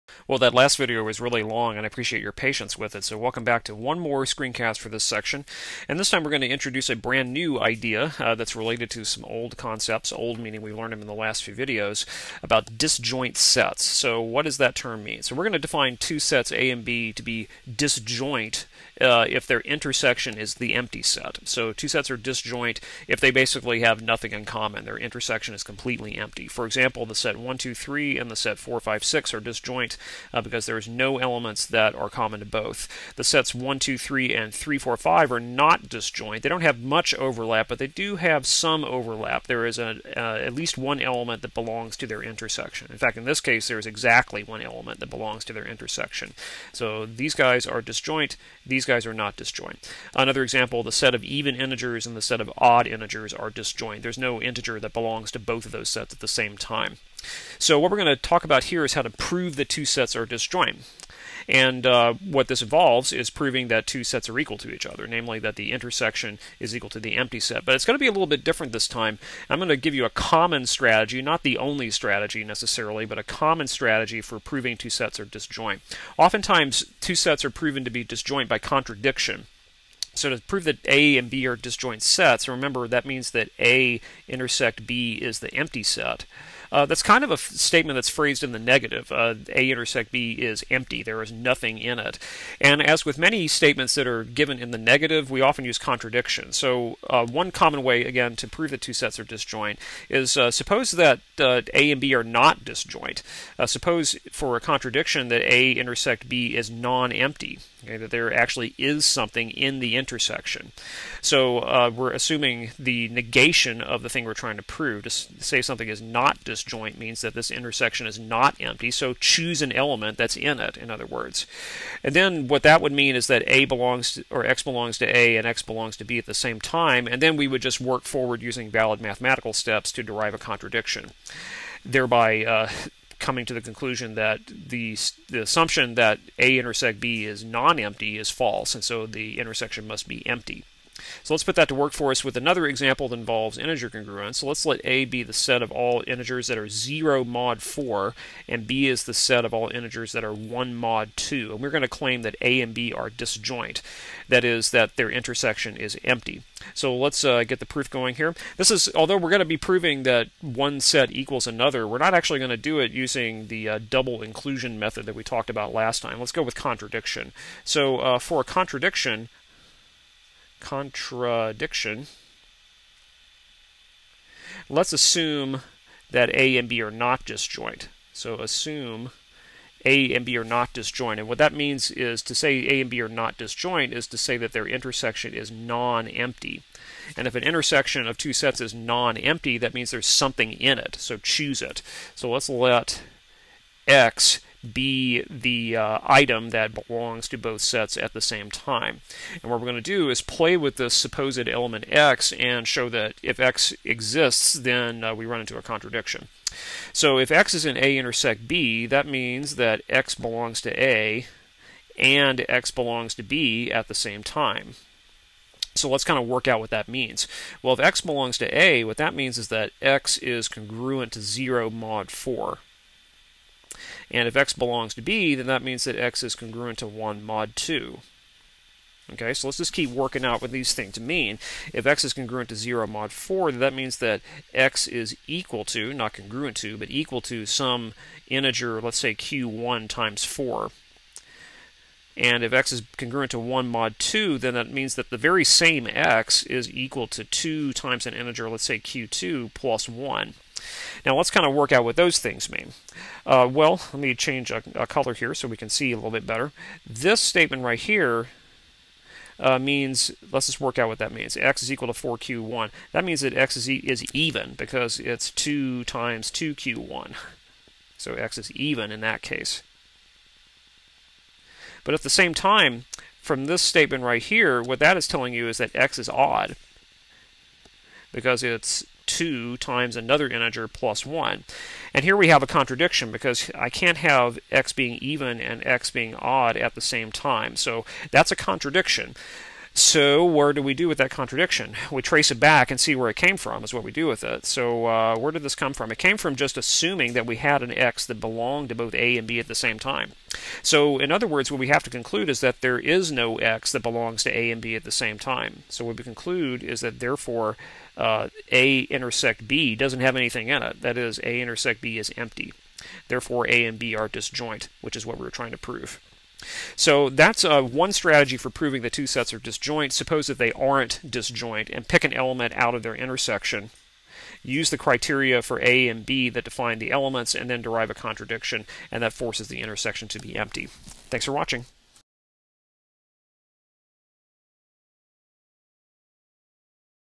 Disjoint sets (Screencast 5.2.3)
This video defines the concept of disjoint sets and gives an example of a strategy for how to prove two sets are disjoint.